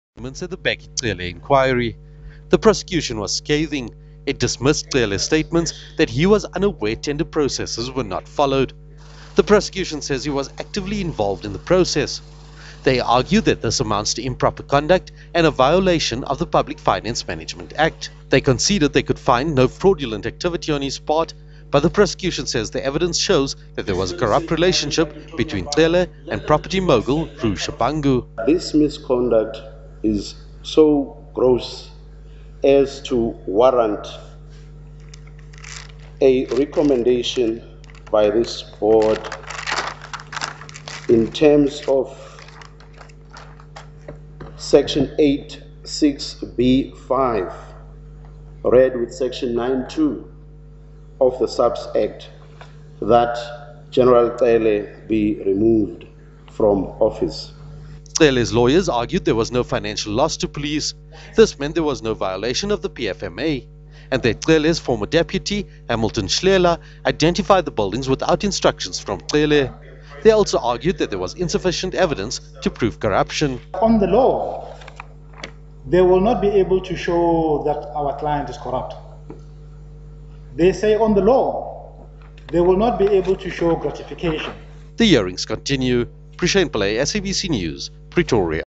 State's legal team recommends Cele's removal
The State's legal team has asked the Board of Inquiry to recommend that suspended National Police Commissioner Bheki Cele be removed from his office...